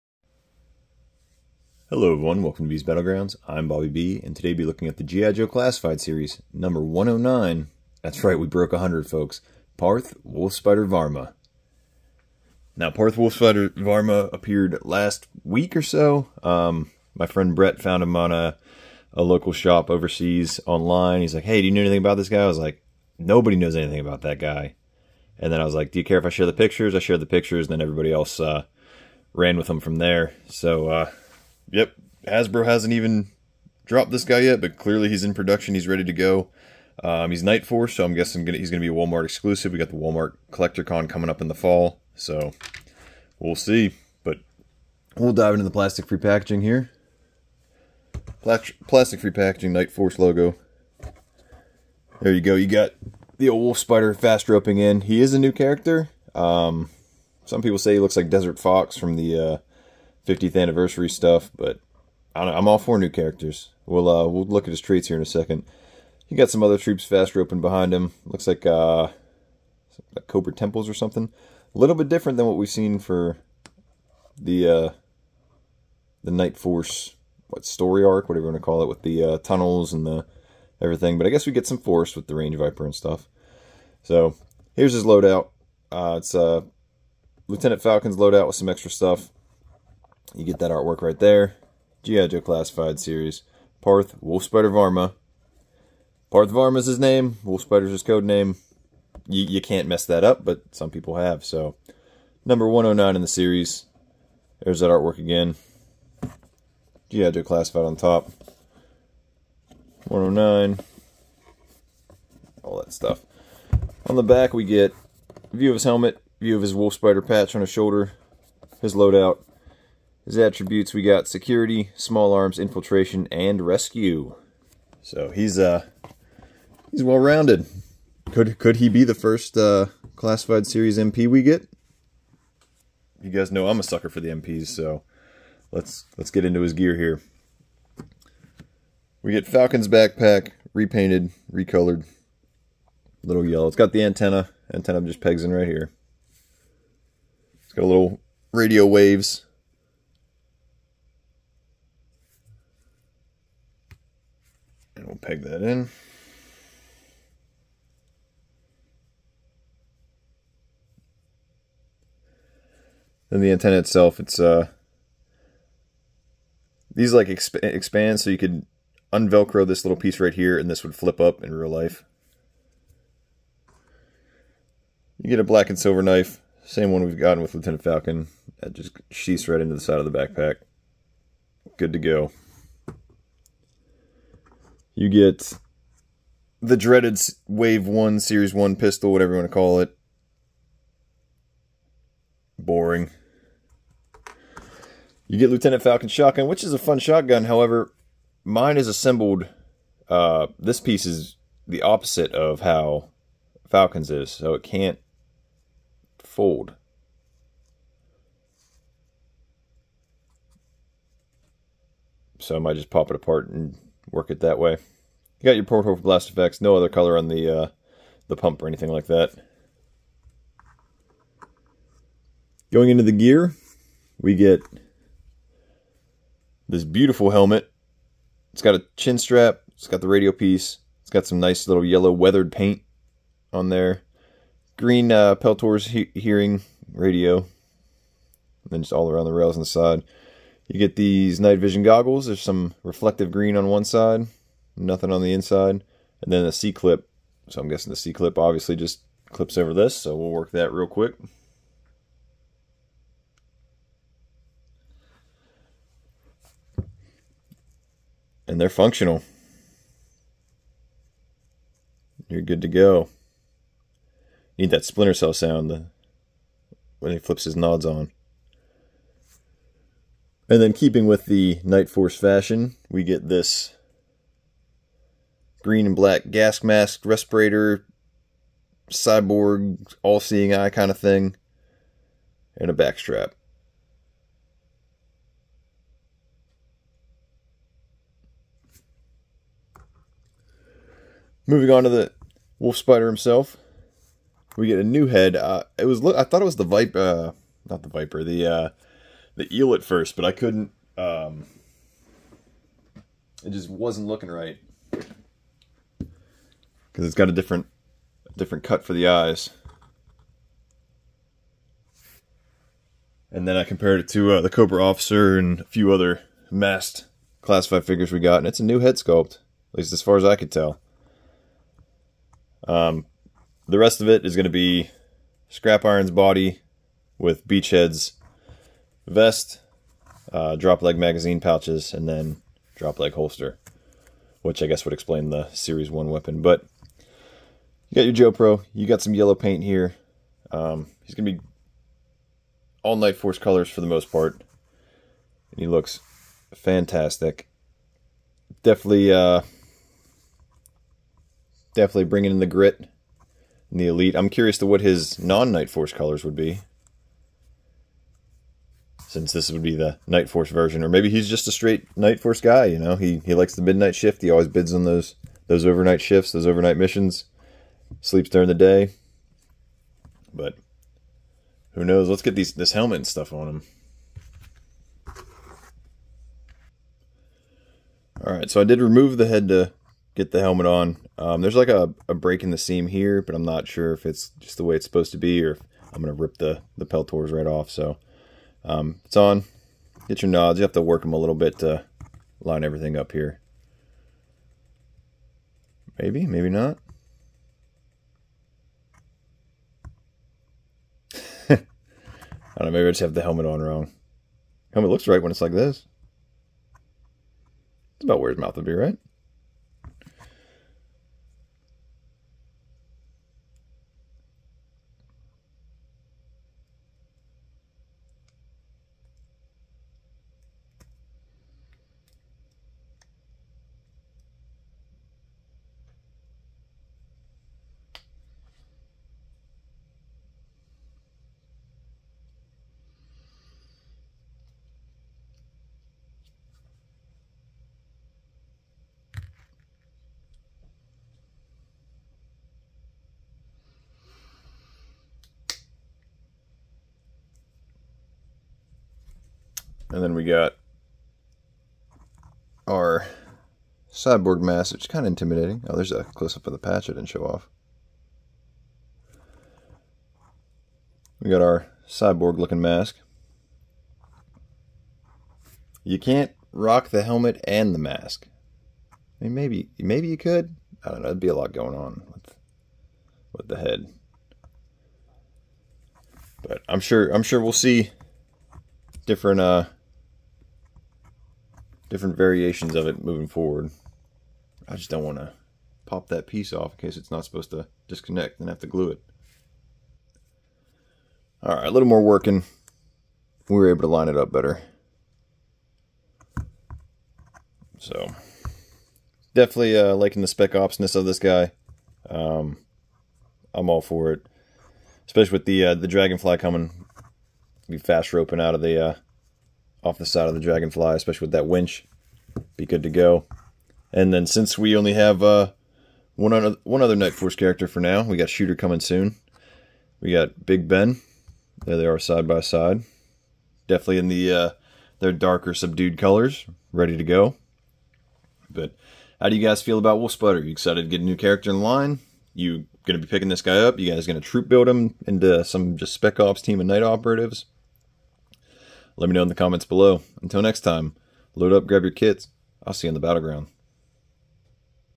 GIJoe Classified Series Parth Wolf Spider Varma Nightforce
A look at the Exclusive GIJoe Classified Series number 109 Nightforce Parth "Wolf Spider" Varma